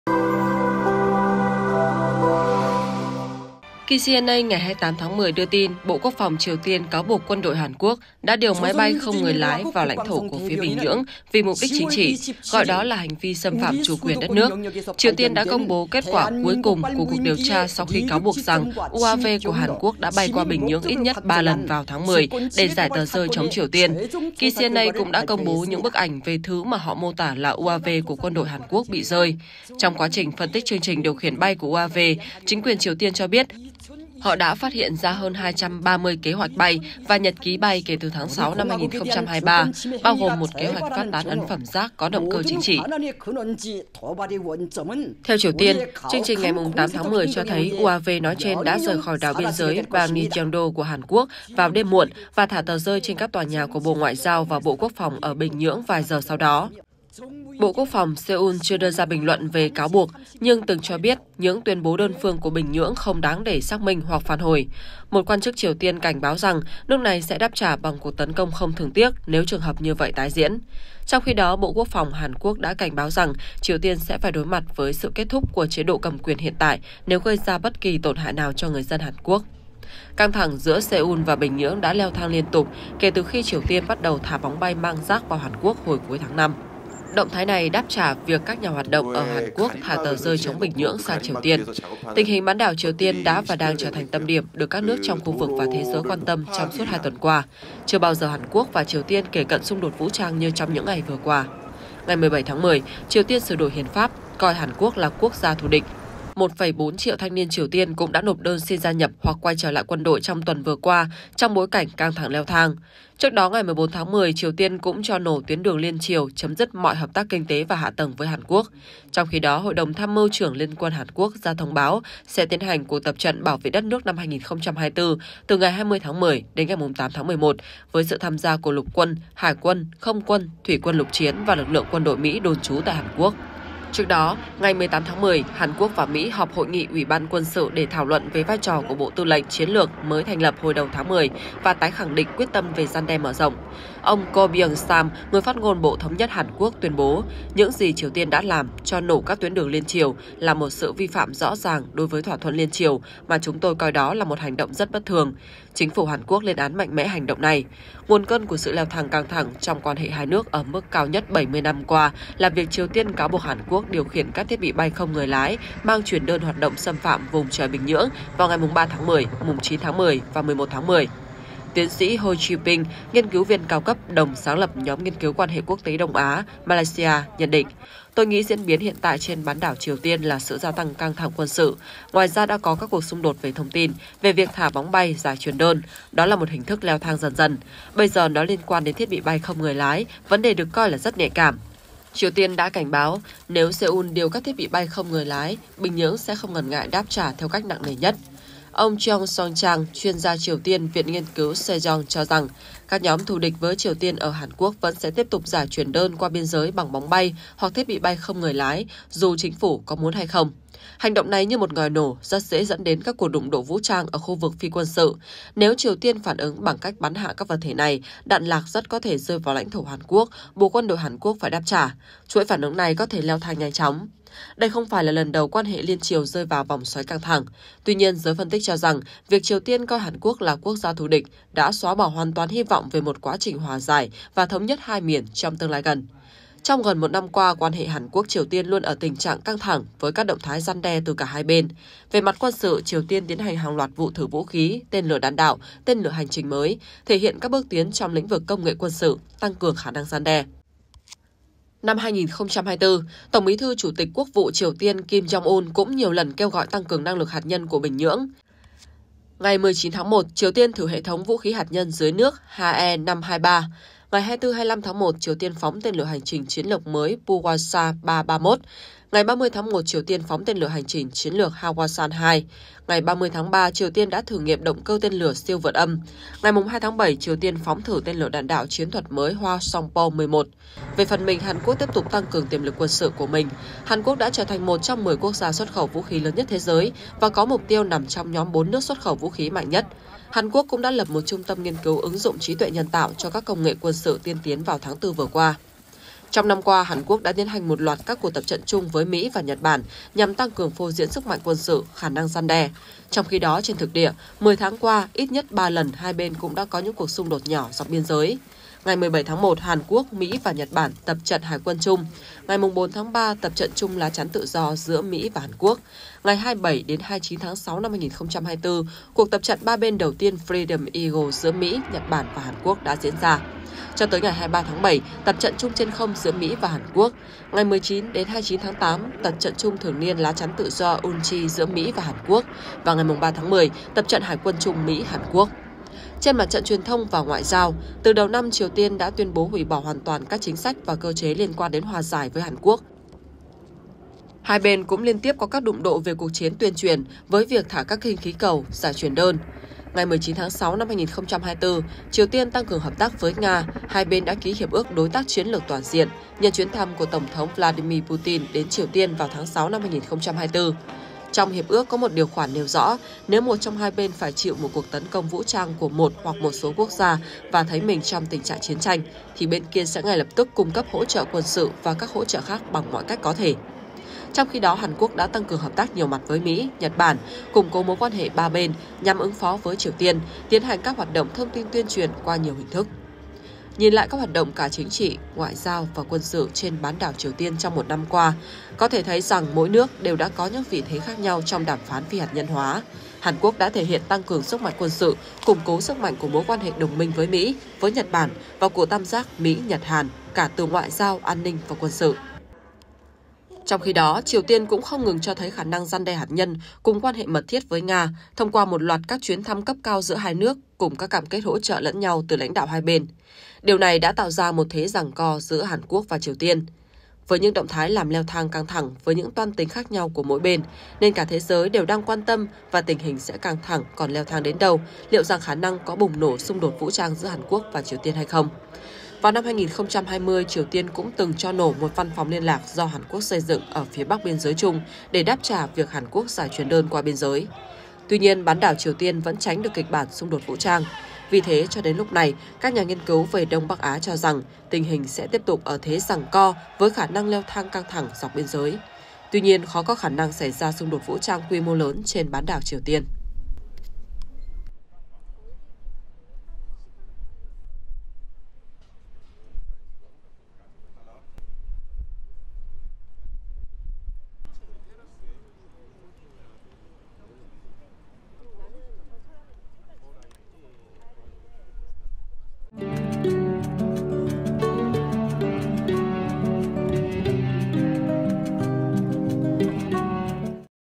Triều Tiên cáo buộc quân đội Hàn Quốc về vụ xâm nhập UAV, tình hình nóng nhất 70 năm qua | SKĐS
SKĐS | KCNA ngày 28/10 đưa tin, Bộ Quốc phòng Triều Tiên cáo buộc quân đội Hàn Quốc đã điều máy bay không người lái bay vào lãnh thổ của phía Bình Nhưỡng vì mục đích chính trị, gọi đó là hành vi xâm phạm chủ quyền của đất nước.